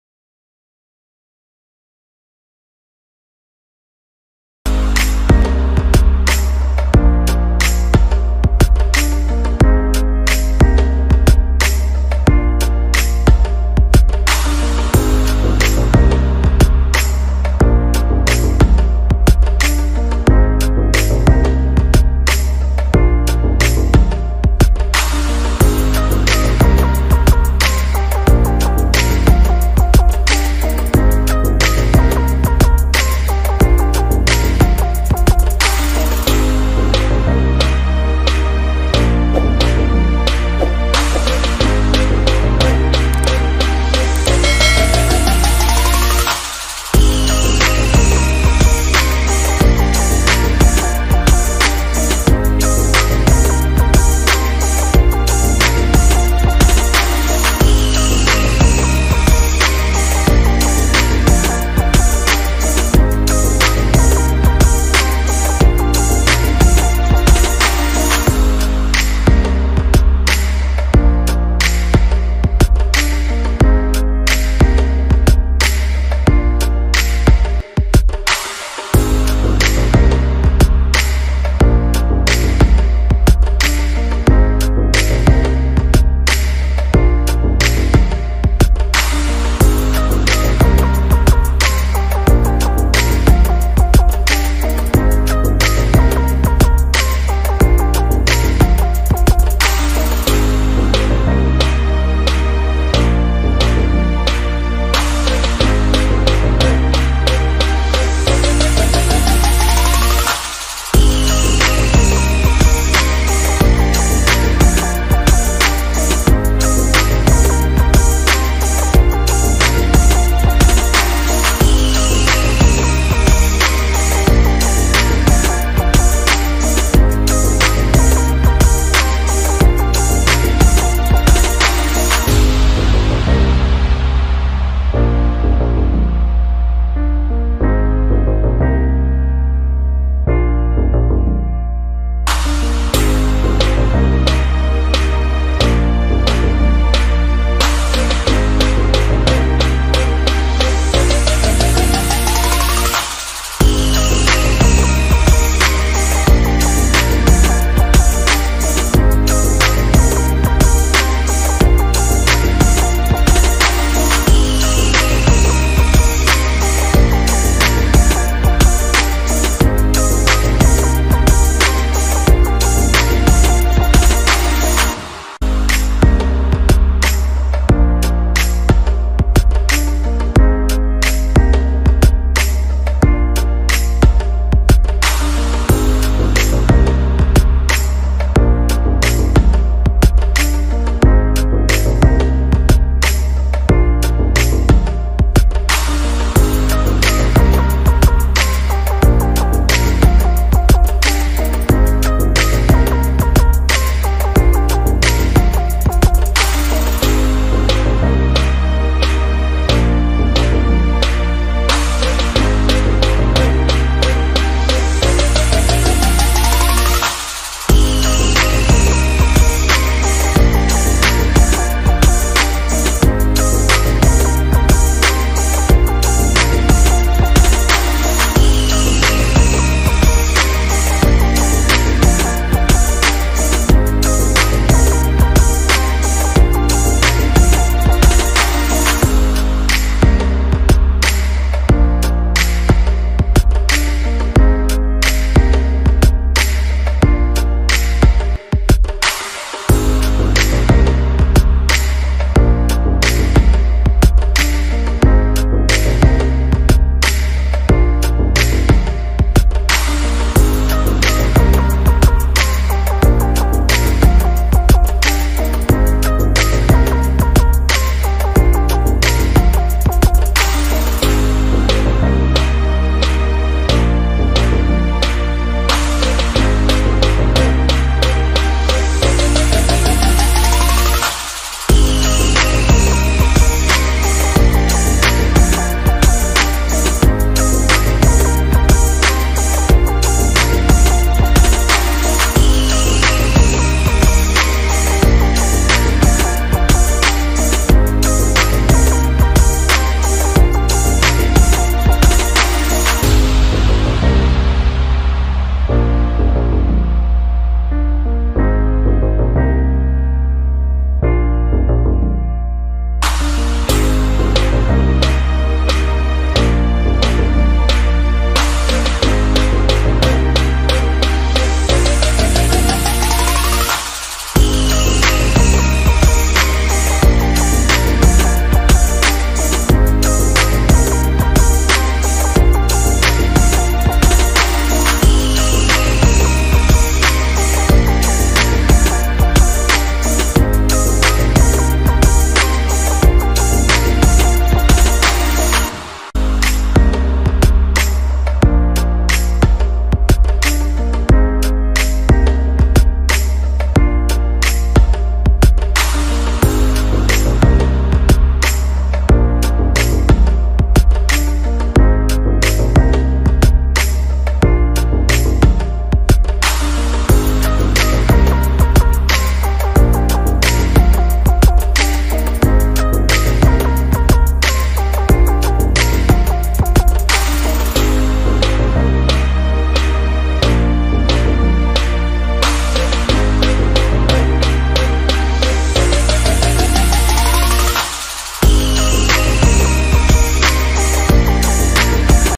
How can make scenery / Easily water paint drawing / easy art / sunset art
In this video make a scenery .using only water paint. easy process every one do it

This types of video link
SAC VIDEO-1